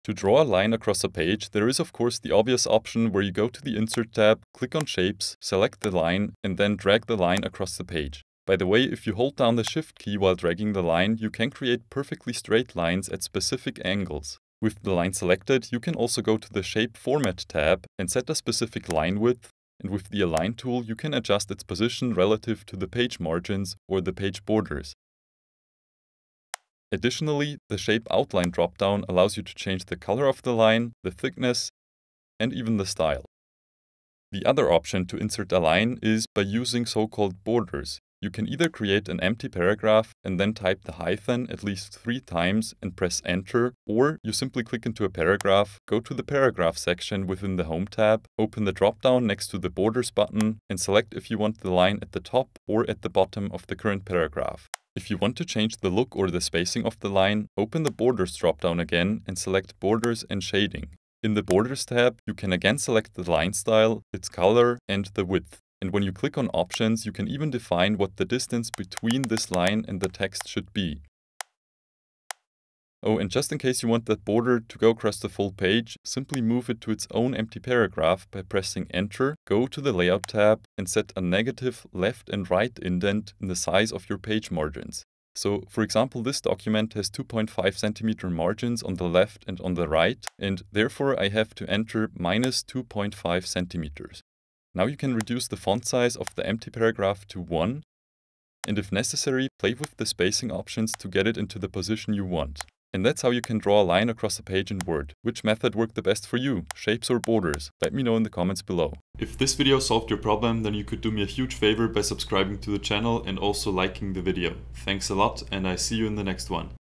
How to Add a Line Across the Page in Word
Did I save your time and nerves by getting right to the point? ❤️

If you want to add a line across the page in Word, you do have multiple options. The most flexible one is inserting a line shape. You will be able to customize almost every part of it. However, using line shapes should be done with care, as the positioning can easily get very inconsistent. Therefore, you will also see a second method for inserting a line across in page and that one offers way more consistency.

⏱ Timestamps:
00:00 Insert a Line SHAPE, Customize and Align It
00:36 Insert a Border & Customize It
01:48 Outro